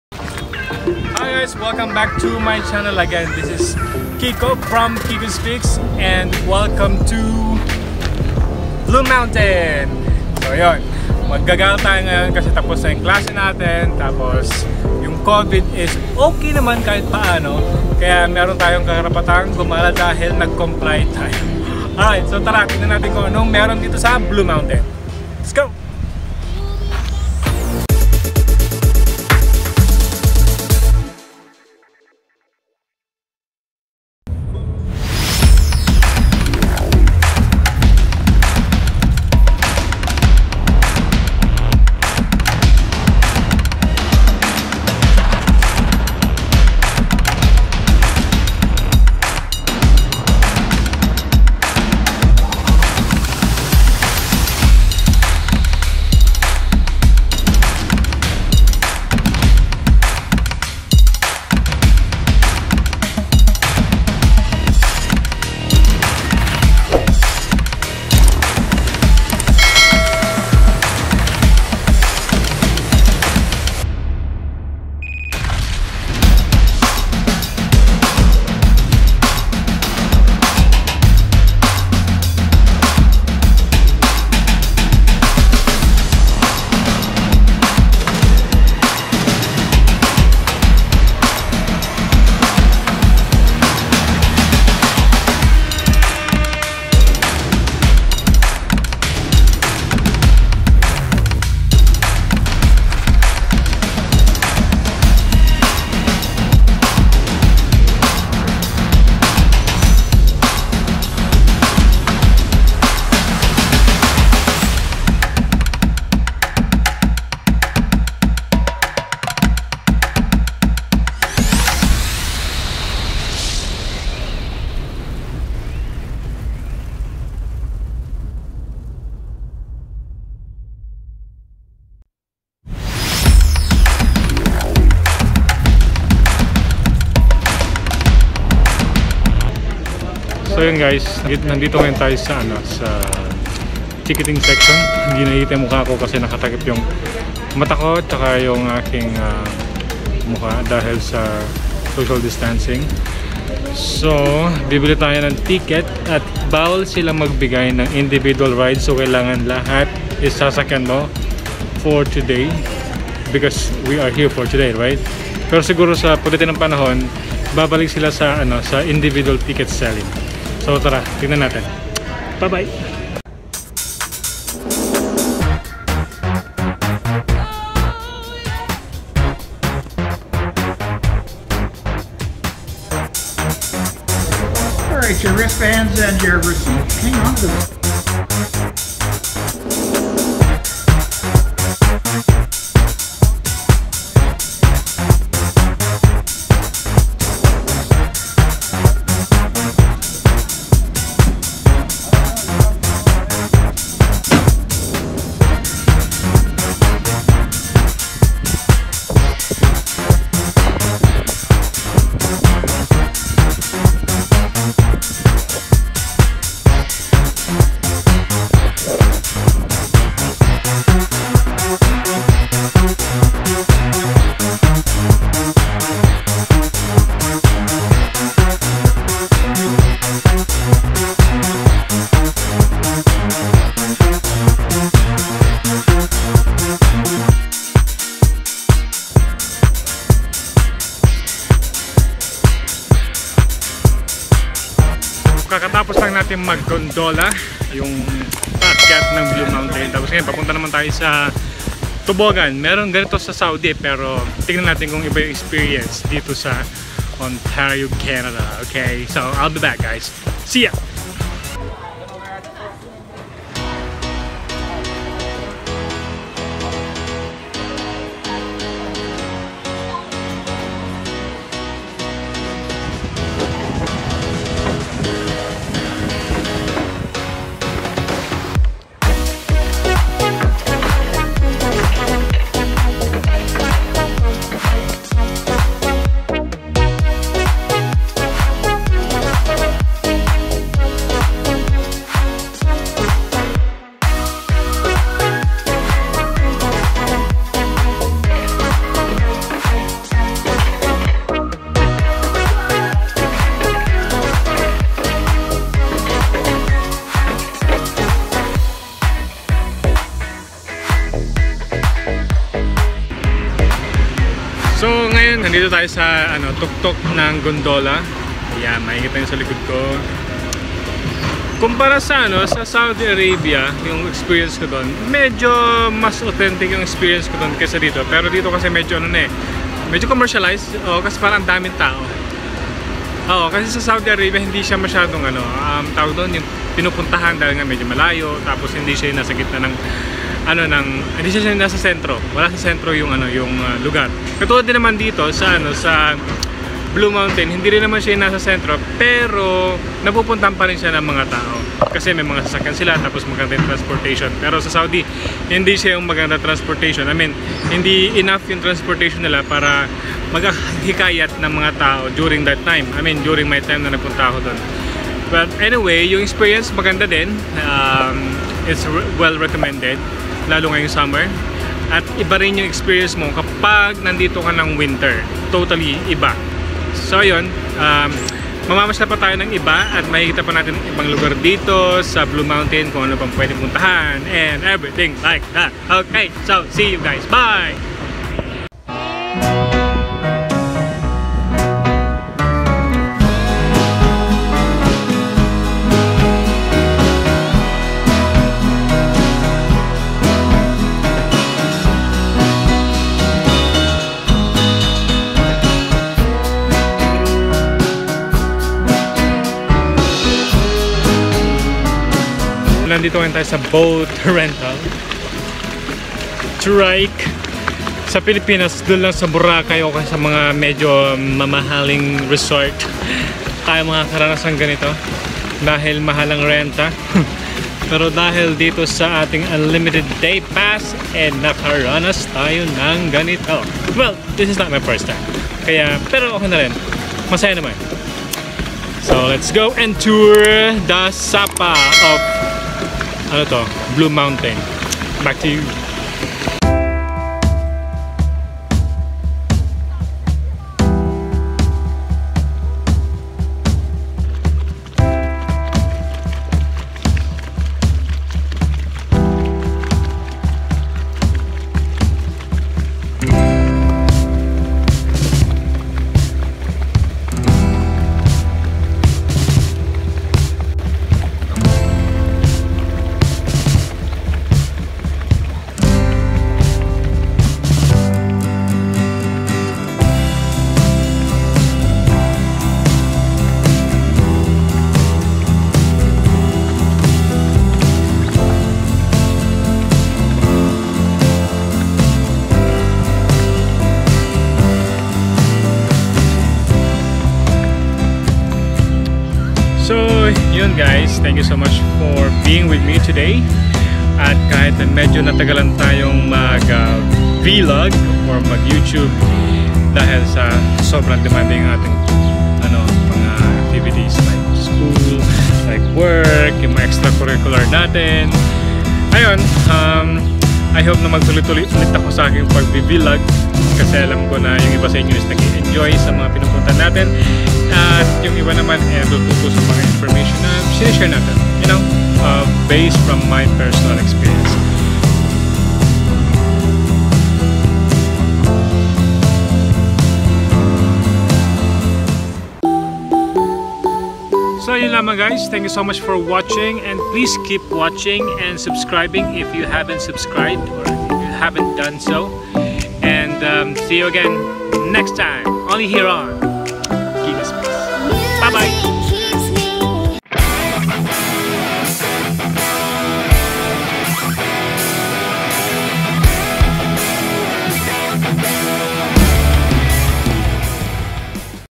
Blue Mountain Experience in Summer | Things To Do in Ontario Canada | Alpine Ski Resort | Not Winter
Here's my video (after a long while) about my trip to Blue Mountain, an alpine ski resort in Ontario, Canada, just northwest of Collingwood. It is situated on a section of the Niagara Escarpment about 1 km from Nottawasaga Bay, and is a major destination for skiers from southern Ontario.

Summer gives a different experience than winter. There are more things to do, rides to enjoy and sceneries to spot.